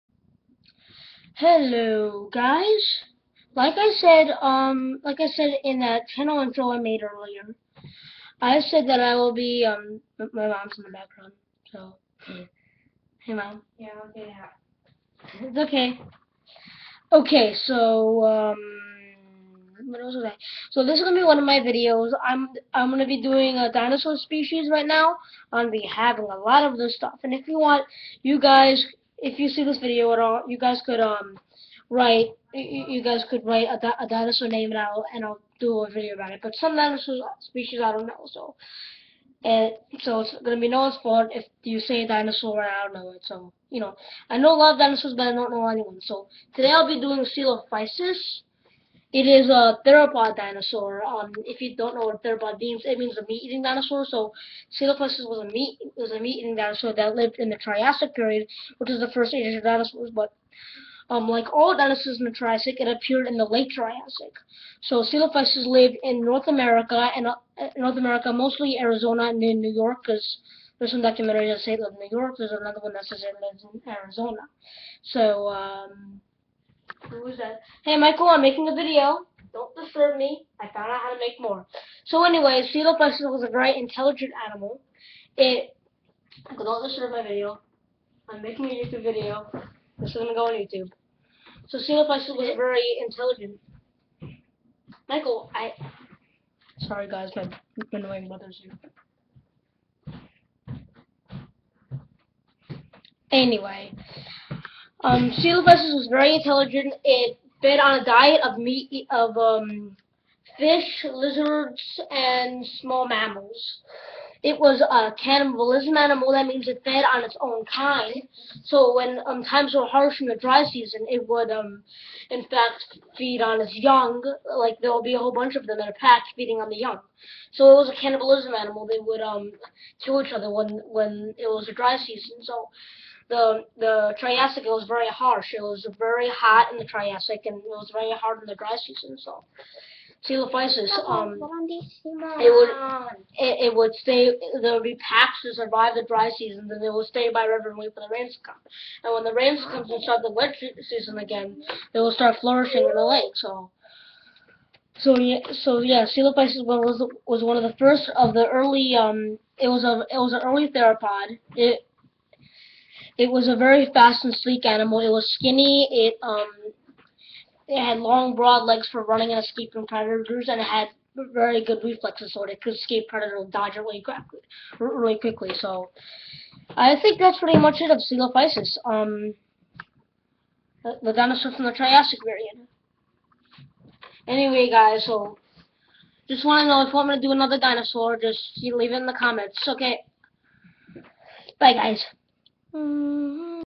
ceolophysus
here is a video on the dinosaur celophysus  sorry my brother got in the way.